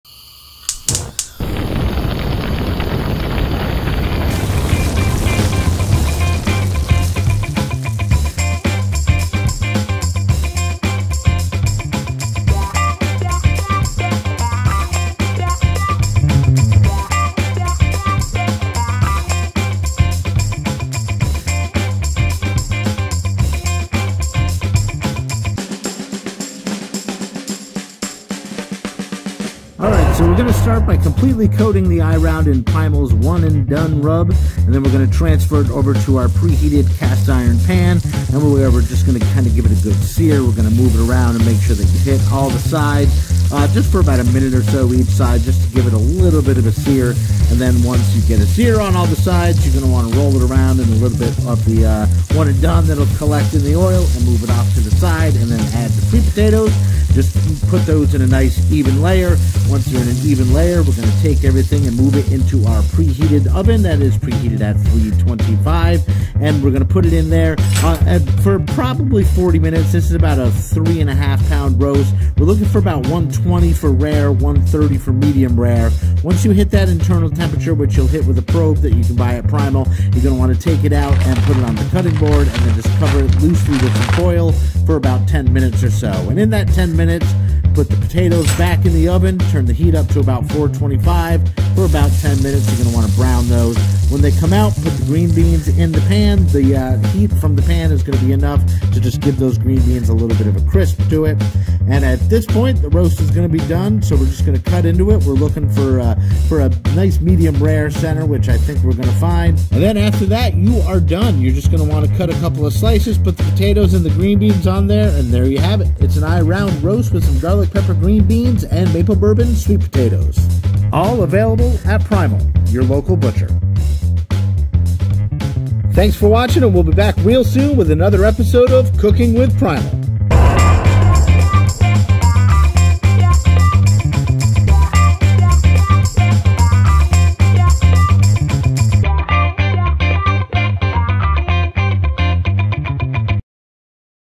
Cooking With Primal Eye Round Roast
On this episode we cook an Eye Round Roast, with Primal's Garliic/Pepper Green Beans and our Maple/Bourbon Sweet Potatoes.